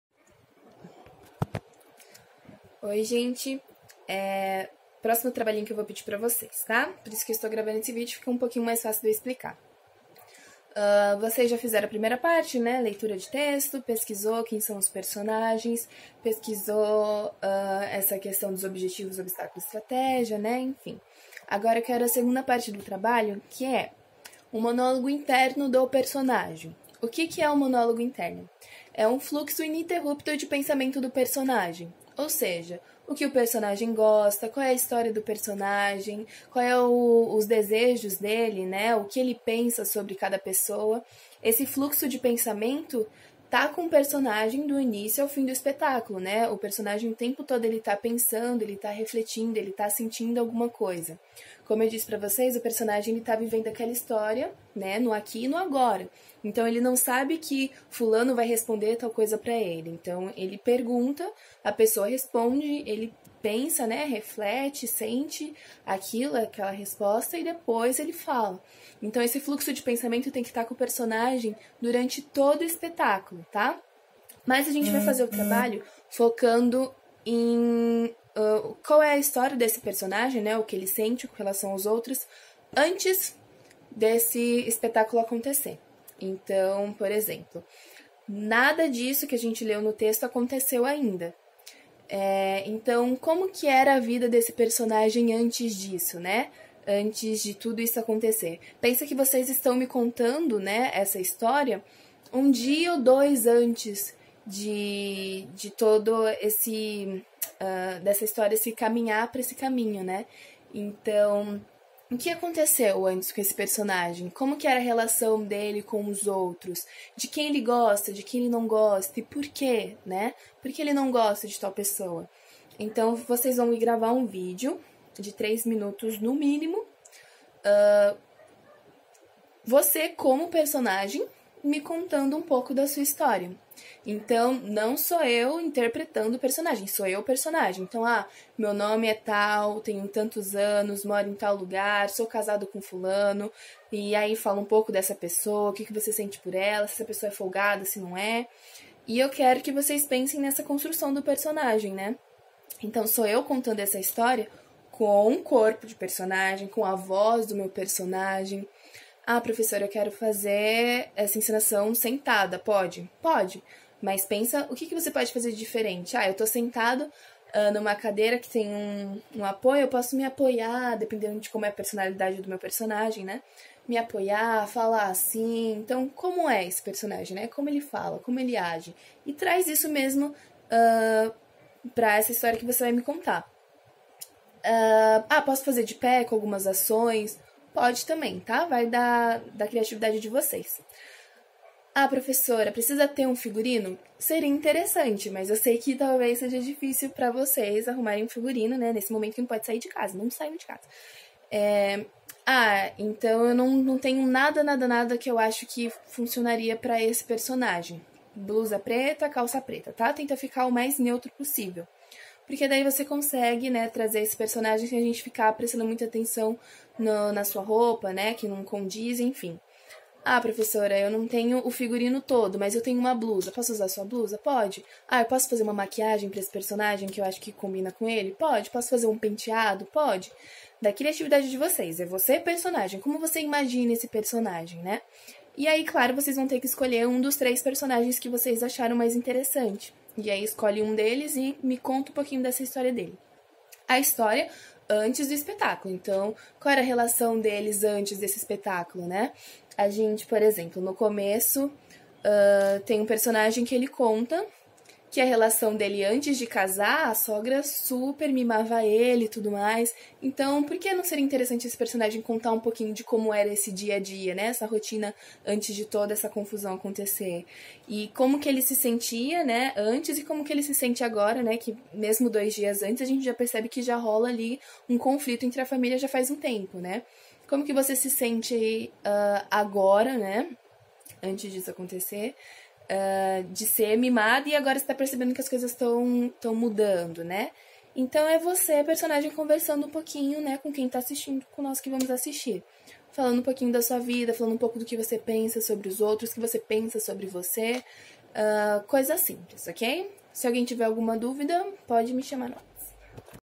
AULA 04 TEATRO MÓDULO 1 MANHÃ E TARDE
Acompanhe a quarto aula de Teatro para os alunos do módulo 1, nos períodos da manhã e tarde, com a professora Stephany Horminio, da Escola Livre de Teatro e Circo.